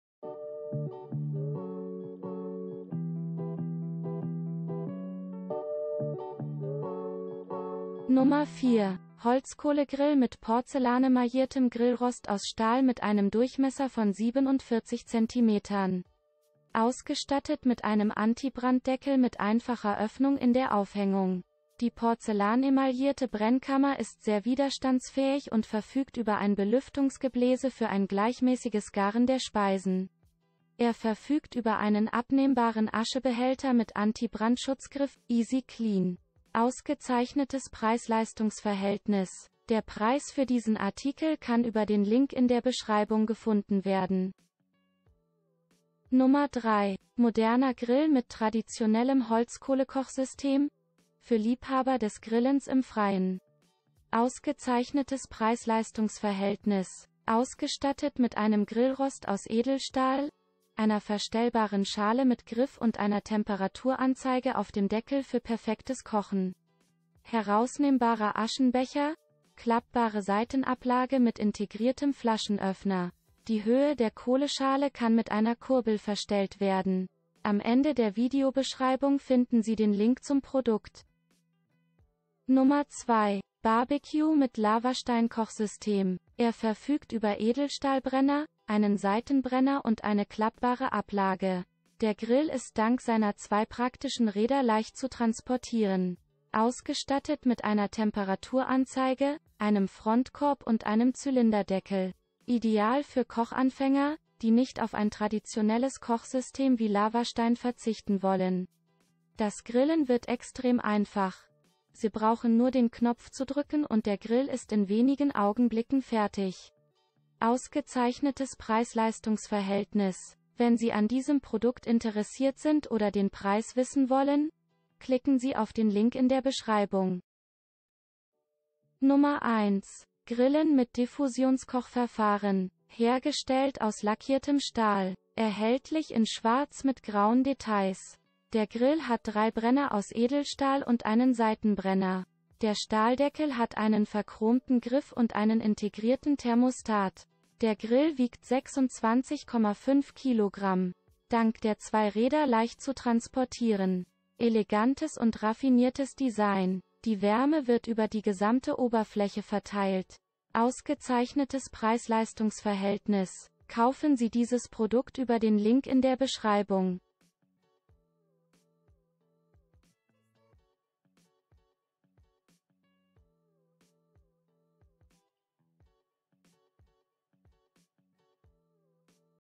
Die 4 besten Sochef-Grills für perfektes Grillen
✋ Hier ist die 4 besten sochef-grills für perfektes grillen, auf dem Markt verfügbar .

 4) Sochef G20040210 Pupo 47 Grill.

 3) Sochef - Salmar – Gringo – Holzkohlegrill.

 2) Sochef Piùsaporillo Grill G20513.

 1) Sochef G32013 DiabloX Barbecue, Streumethode, Stahl lackiert...

Zeitstempel (Timestamps)

00:00 - Einführung
00:08 - 4) Sochef G20040210 Pupo 47 Grill.
00:46 - 3) Sochef - Salmar – Gringo – Holzkohlegrill.
01:22 - 2) Sochef Piùsaporillo Grill G20513.
02:08 - 1) Sochef G32013 DiabloX Barbecue, Streumethode, Stahl lackiert...